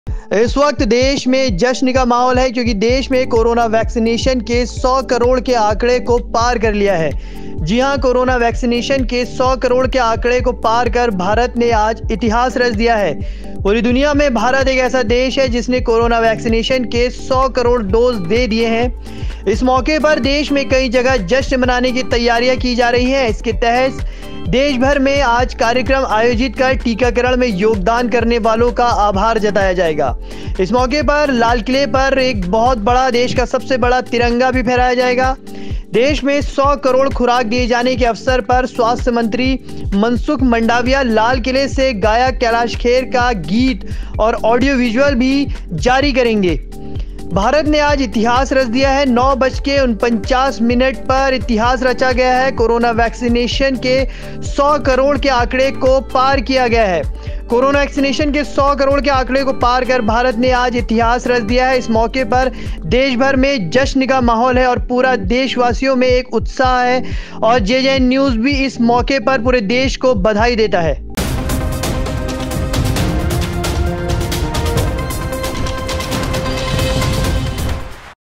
आज भारत ने रच दिया इतिहास कोरोना टीकाकरण का आकड़ा पहुंचा 100 करोड़ के पार || JJN News
Research/Script - Mohd Uvesh
VO - Mohd Uvesh
Editor - Mohd Uvesh

JJN News Nainital,
JJN News,